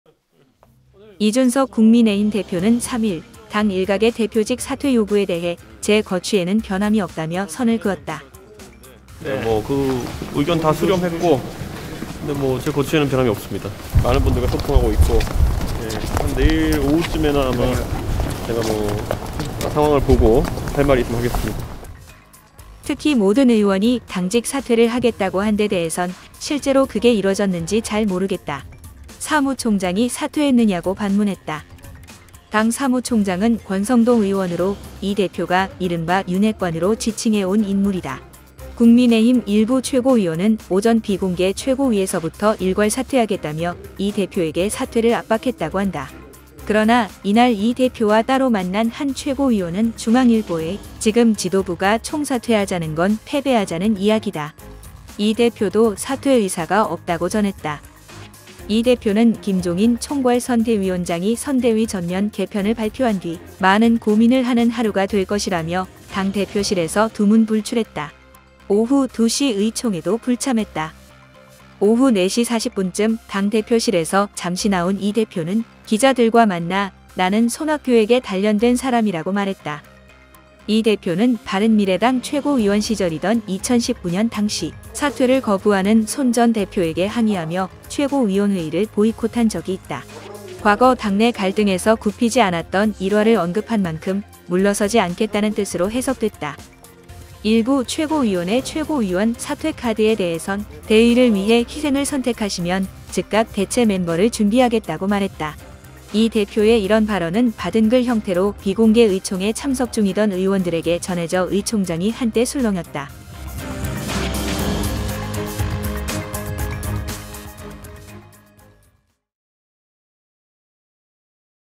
"나는 손학규에게 단련된 사람" 줄사퇴 와중에…'대표직 사수' 내비친 이준석
국민의힘 중앙선거대책위원회에 ‘폭풍우’가 몰아친 3일 이준석 국민의힘 대표는 “제 거취에는 변함이 없다”며 물러날 뜻이 없음을 분명히 했다.

이 대표는 선대위를 둘러싼 상황에 대해 이날 오전부터 “제 의사표시를 자제하겠다”며 말을 아꼈다. 김종인 총괄선대위원장이 중앙선거대책위원회의에서 “선대위 전면 개편”을 발표한 직후 기자들과 만나선 “언론이 아는 내용 이상으로 알고 있지 못하다. 어떤 형태로 최종 귀결될지 아직 모르기 때문에 평가나 의사표시를 자제하겠다”고 말했다. 이어 “저 역시도 오늘 많은 고민을 하는 하루가 될 것”이라고 덧붙였다.

이 대표는 이날 당초 참석 예정이었던 2시 당 의원총회에도 불참했다. 전직 사무총장인 한기호 의원과 오찬을 함께 한 뒤부터는 당 대표실에서 두문불출했다.

그러나 당내에선 제기된 ‘당 대표 사퇴’ 요구에 대해선 선을 그었다.